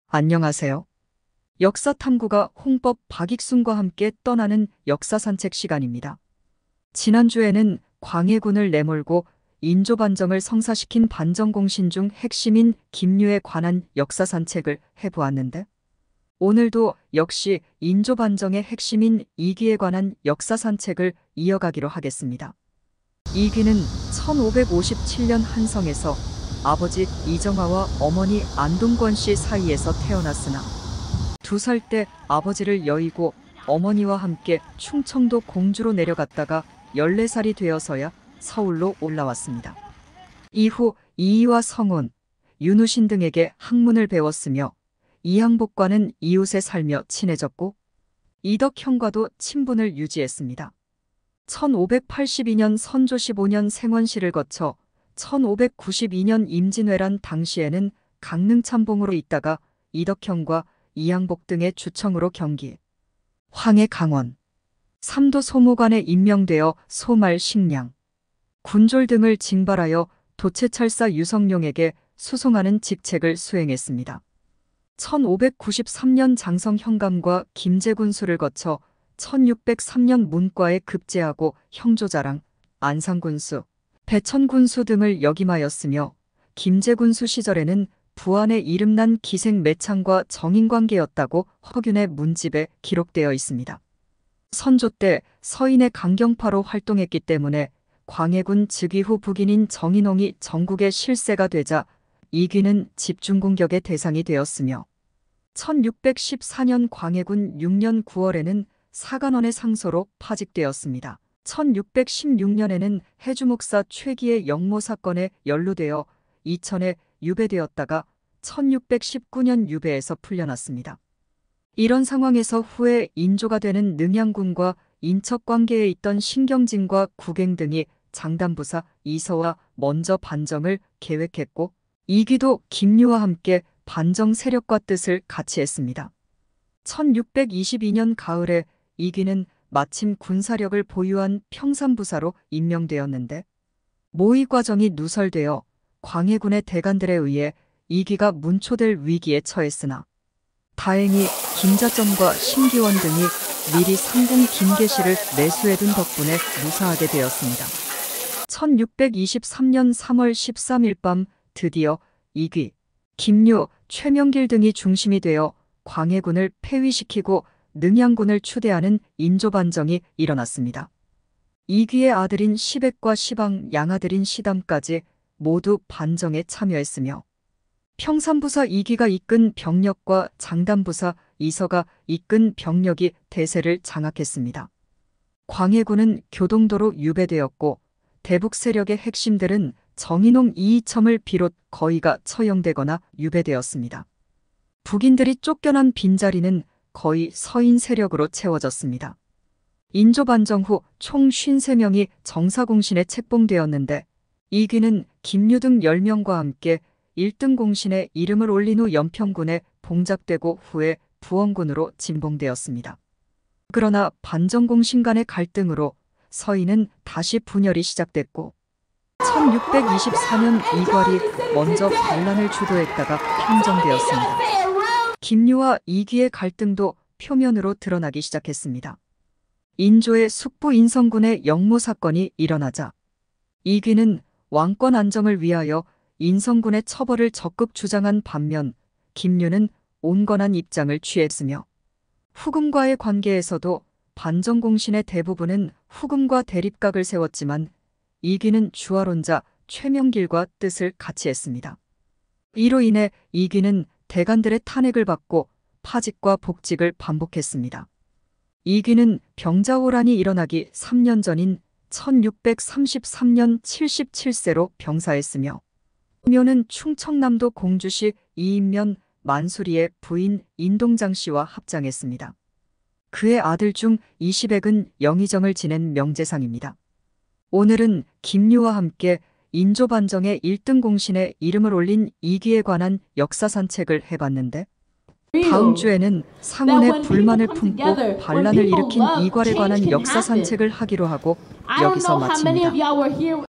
인조반정의 일등공신 이귀
광해군을 내몰고 인조반정을 일으킨 이귀는 김류 신경진등과 능양군을 추대하여 인조반정에 성공했지만 반정공신간 대립과 갈등이 지속된다.
이귀는 최명길과 함께 대표적인 주화론자로 청나라와 화친을 주장했으며 병자호란이 일어나기 3년전 병사했다